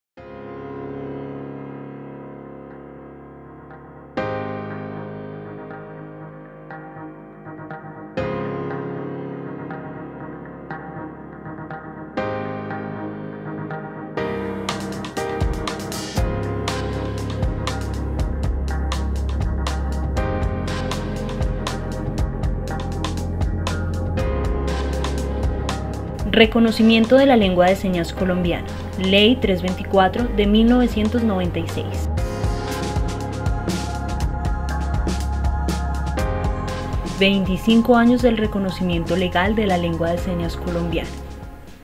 LSC25AÑOS
25 Años del reconocimiento de la Lengua de Señas Colombiana, por medio de la Ley 324 de 1996, resultado de la lucha, enseñanza y promoción por  parte de la comunidad sorda.